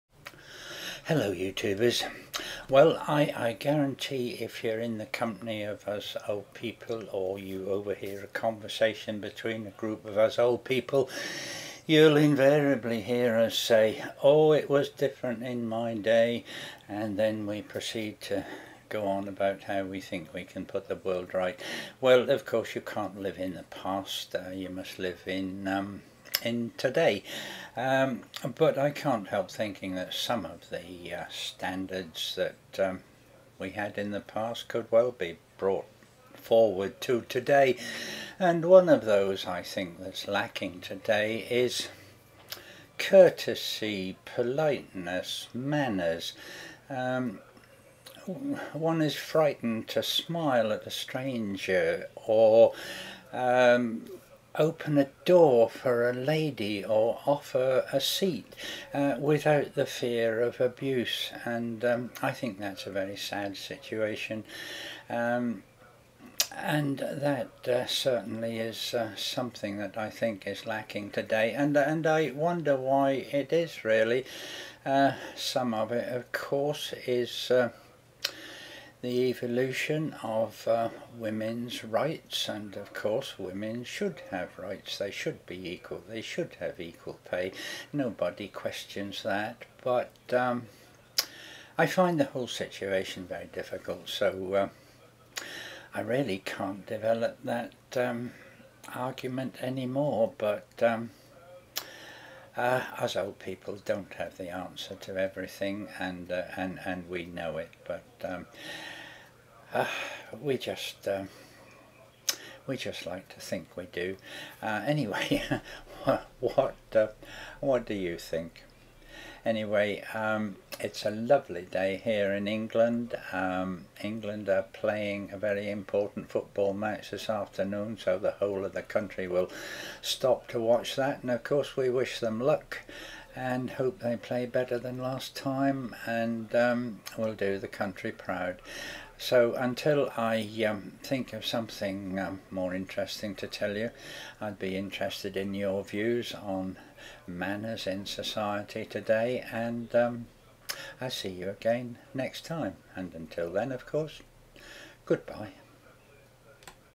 Manners
Is courtesy, politeness and manners absent in today;s society and if so why?